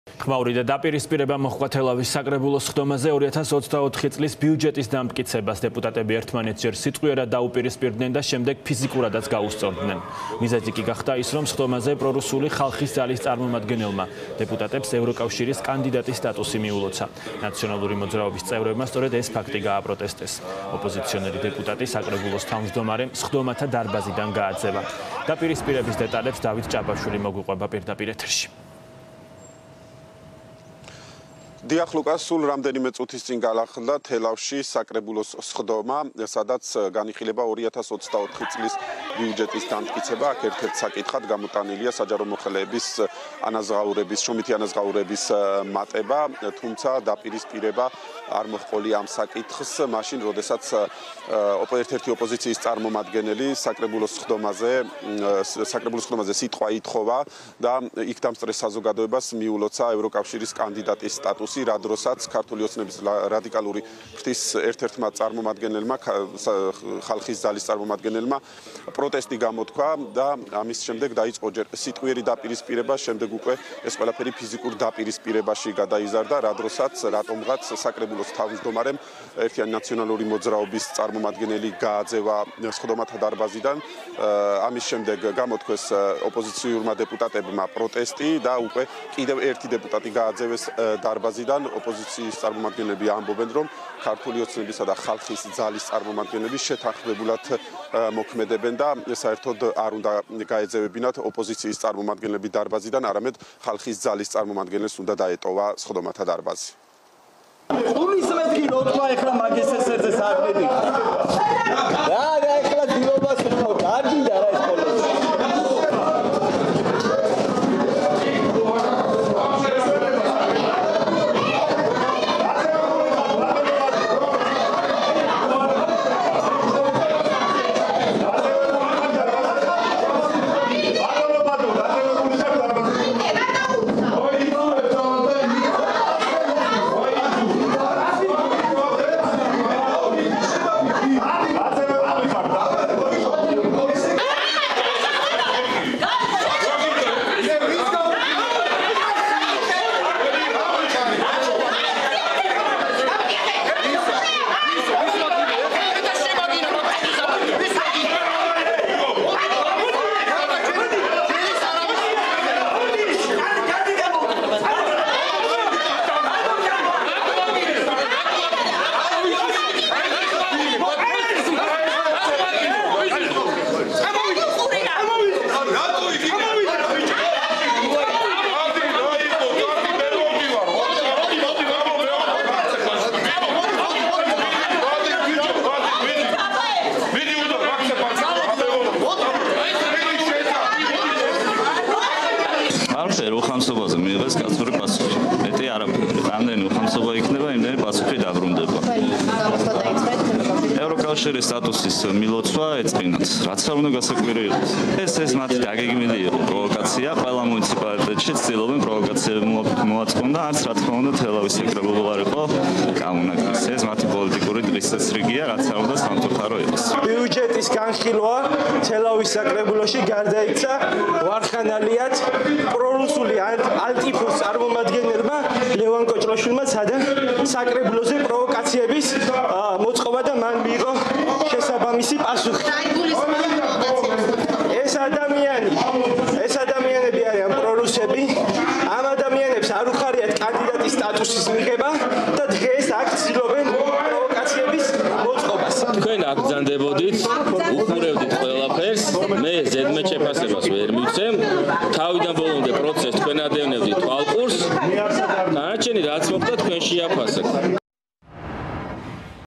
ხმაური და დაპირისპირება თელავის საკრებულოს სხდომაზე
▪️ ხმაური და დაპირისპირება მოჰყვა თელავის საკრებულოს სხდომაზე 2024 წლის ბიუჯეტის დამტკიცებას. დეპუტატები ერთმანეთს ჯერ სიტყვიერად, შემდეგ კი ფიზიკურადაც დაუპირისპირდნენ